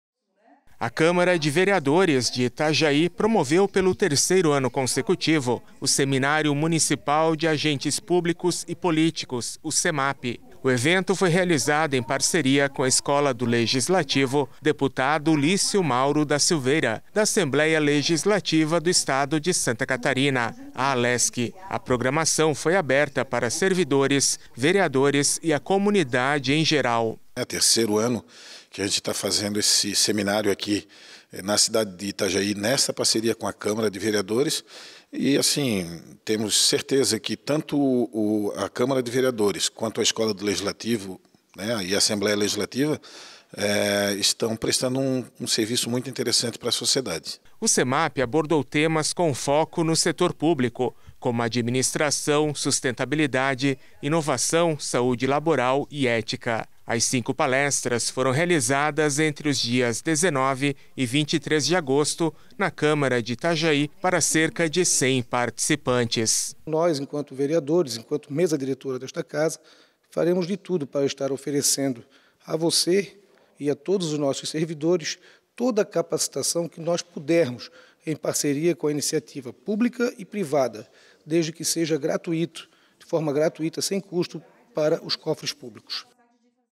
Semapp reúne cerca de 100 participantes na Câmara de Itajaí
Servidores da Câmara de Itajaí participaram do Seminário Municipal de Agentes Públicos e Políticos (Semapp) entre os dias 19 e 23 de agosto. O evento também foi aberto para a comunidade e reuniu cerca de 100 participantes. Foram cinco palestras sobre o Setor Público no Plenário Vereador Arno Cugnier.